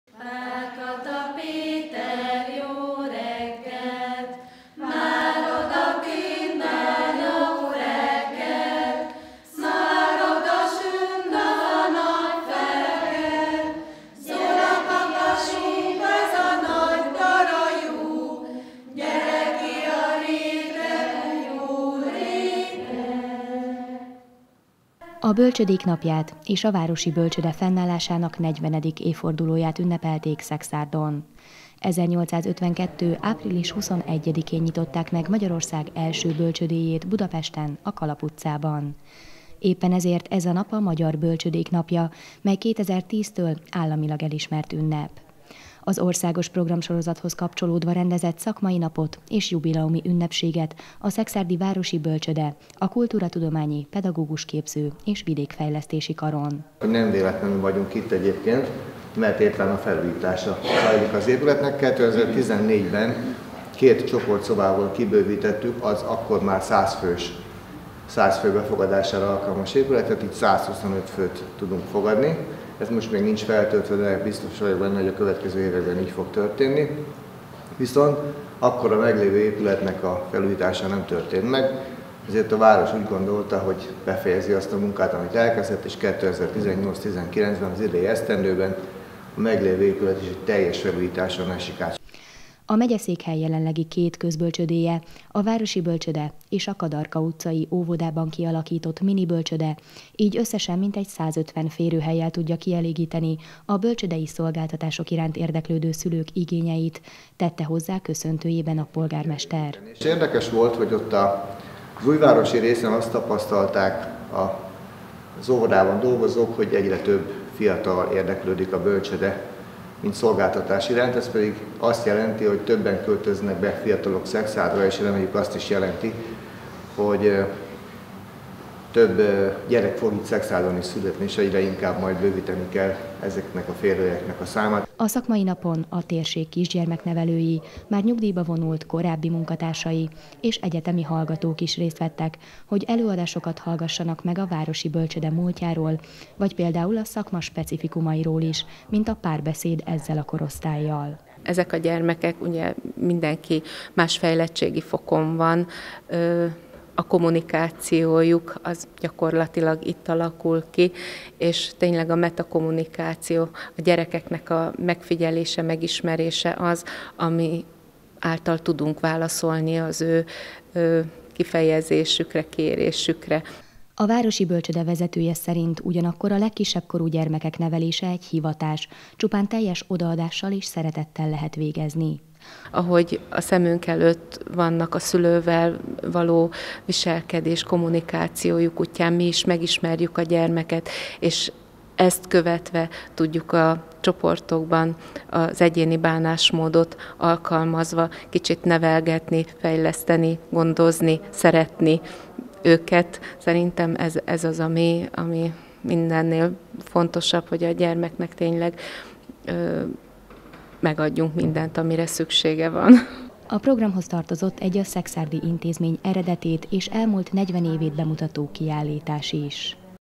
Bölcsődék Napja - Szekszárd 20190425
Április 21-e a Bölcsődék Napja Magyarországon, melyhez kapcsolódóan országszerte rendeznek eseményeket. Szekszárdon idén ezt a Városi Bölcsőde megépülésének 40 éves jubileumával összekötve, egy szakmai nap keretében tartották.